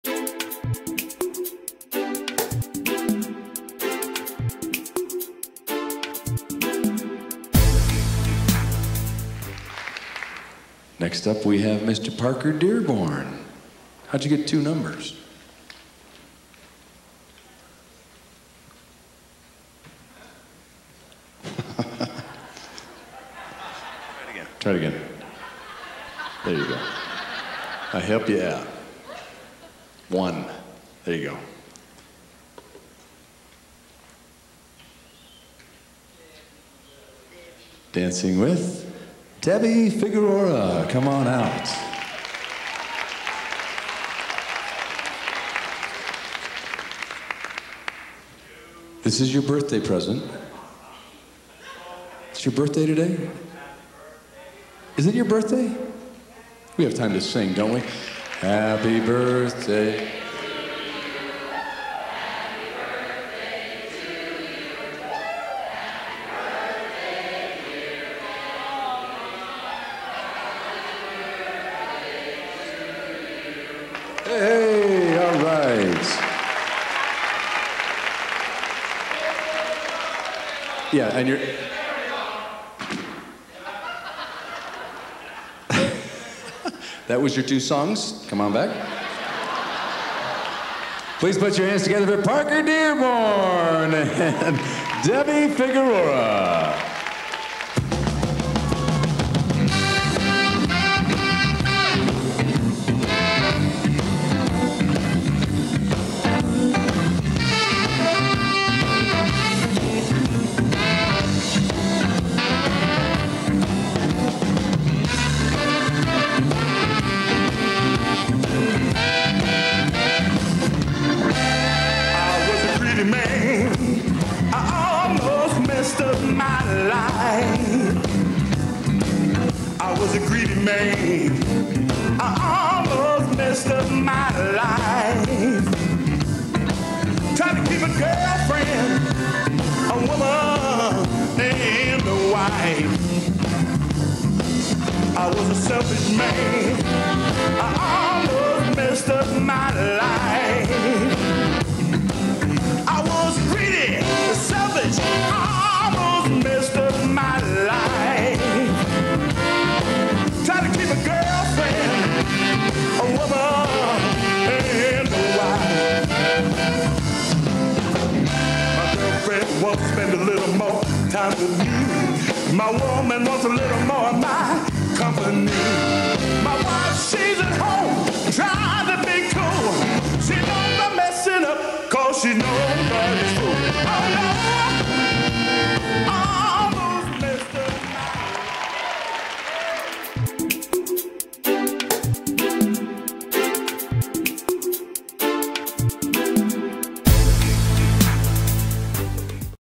Parker Dearborn | Debbie Figueroa | Song 1 | Invitational Jack & Jill 2002 Swingtime in the Rockies
Please "follow" and "like" the Colorado Country Classic on Facebook and Instagram
1. Jordan Frisbee | Tatiana Mollmann
2. Angel Figueroa | Sharlot Bott
3. Kyle Redd | Sarah Vann Drake
4. Wayne Bott | Sylvia Sykes
5. Parker Dearborn | Debbie Figueroa